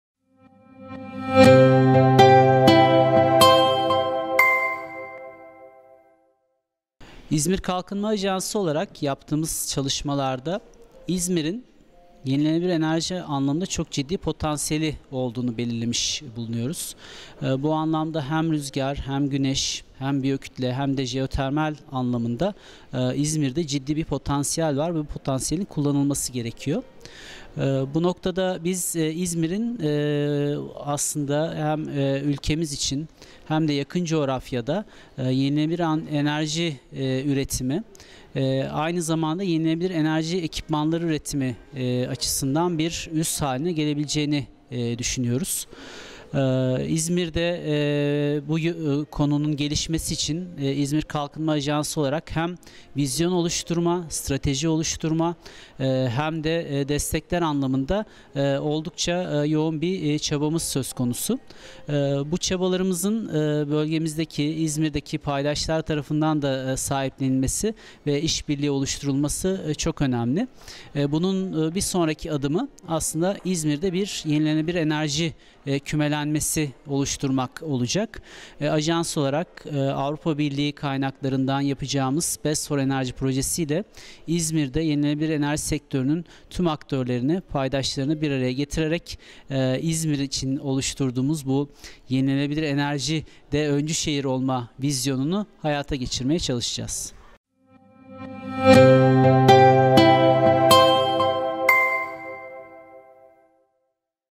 SV2018-Murat Çelik - İZKA
İzmir’in temiz enerjide geleceğini nasıl görüyorsunuz?